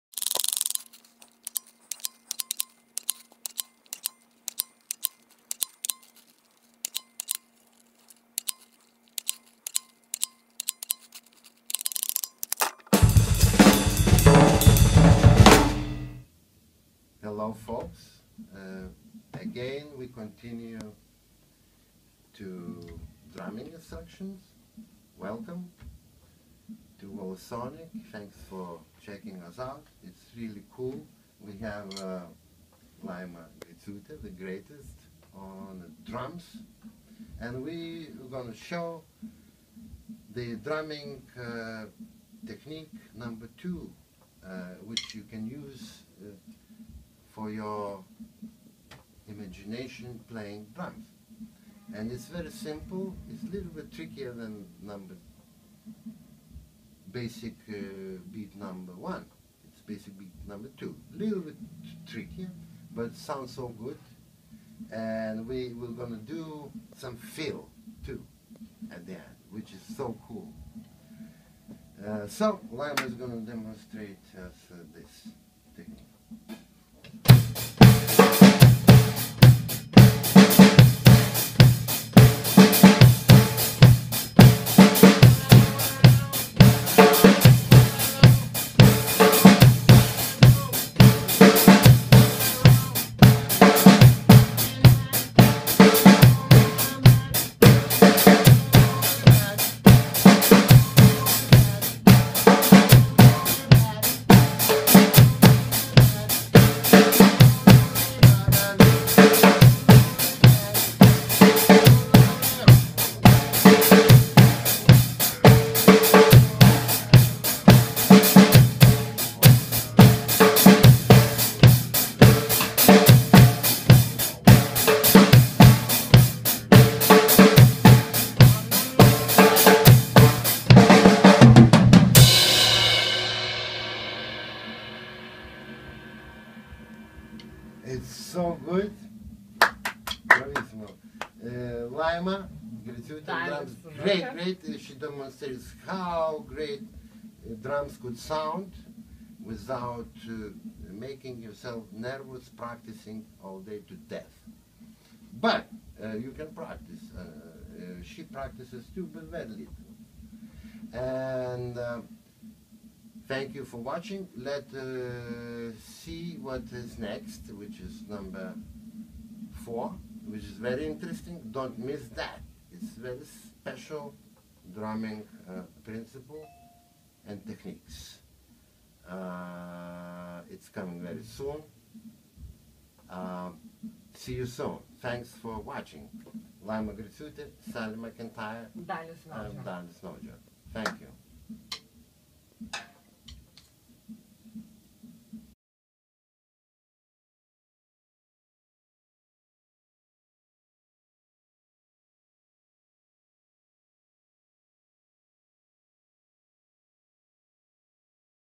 NAUJO DRUMMING INSTRUCTION Series PART 3
Learn to play the drums with Dalius Naujo and Laima.